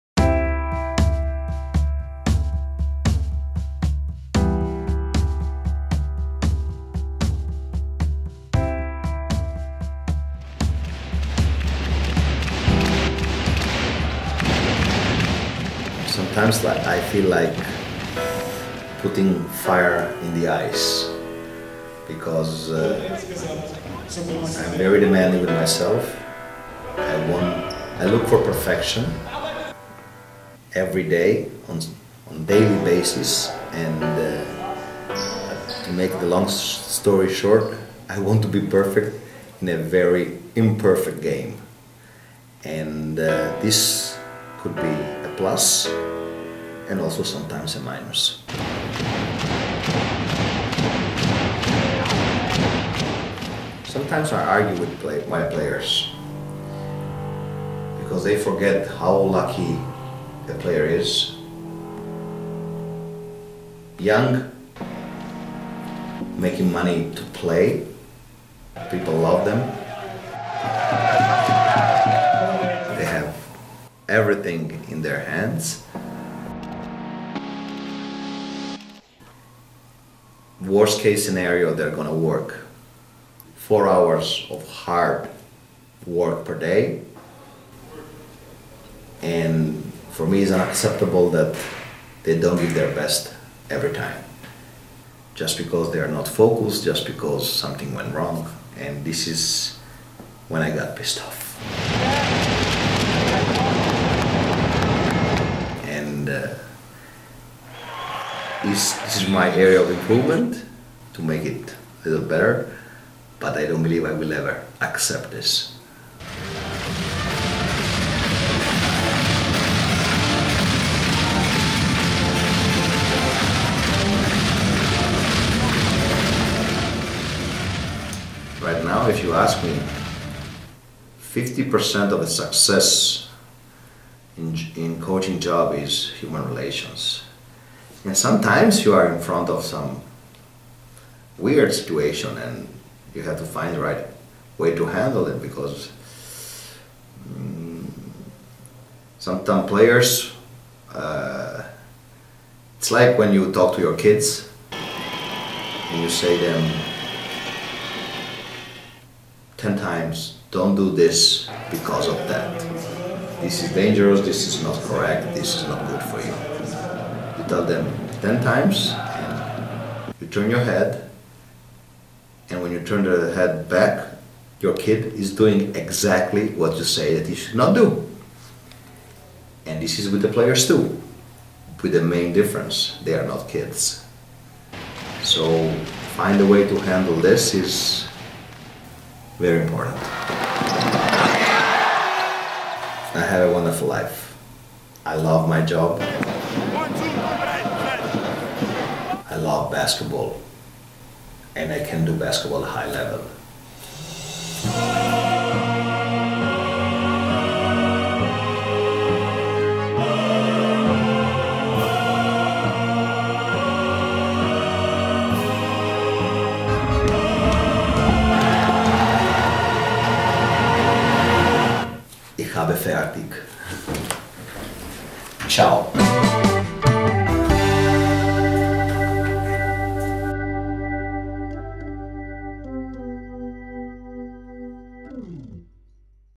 ANDREA TRINCHIERI - the special one in basketball coaching
Portrait über einen besonderen Basketballcoach
Saison 2015 und 2016 - Dauer 4 Minuten
Regie und Schnitt: Christian Schöfer